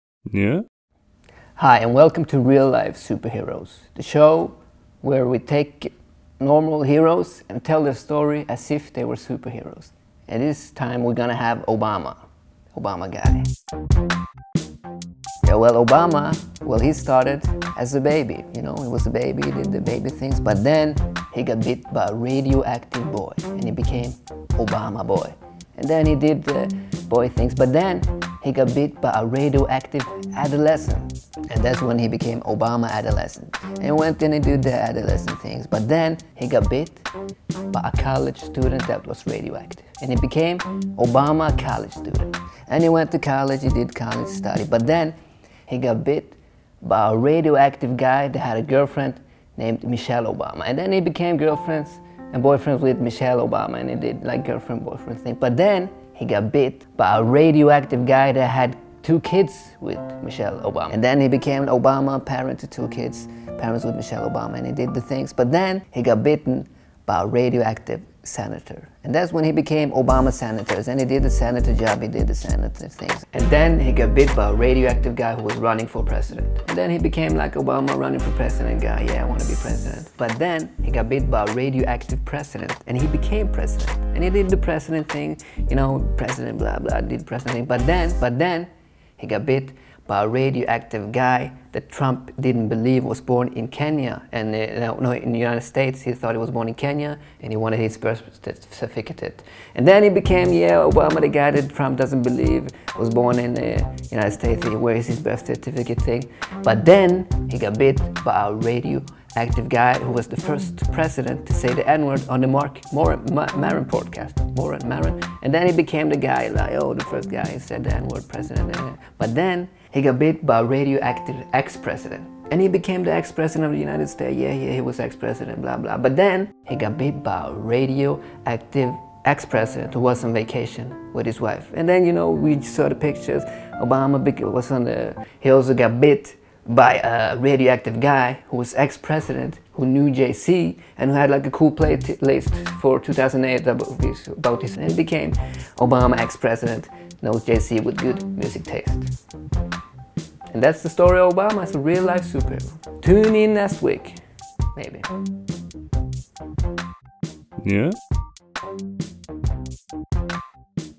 Real Life Superheroes: Obama Guy (Amazing Story!!)
Check out this AMAZING STORY about Obama's life, told as if he was a real life superhero!!

Manus: Francisco Briceno
Foto/Lyd: Jonas Thorstensen
Klipp: Vegar Tryggeseid
Thanks to: Obama life